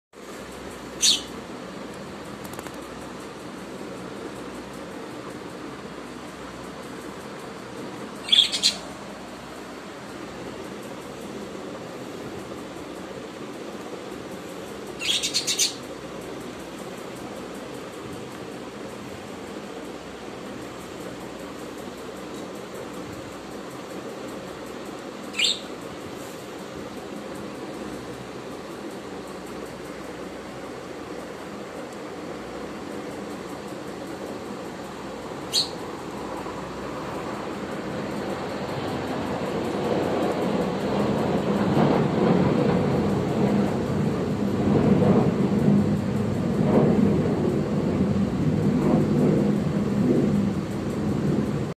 Pet rat eating birds food and bird is screaming 😂 to stop her
Benzi the pet rat eating food of sky the bird 🐦 so she is furious about it saying Benzi to stop eating her food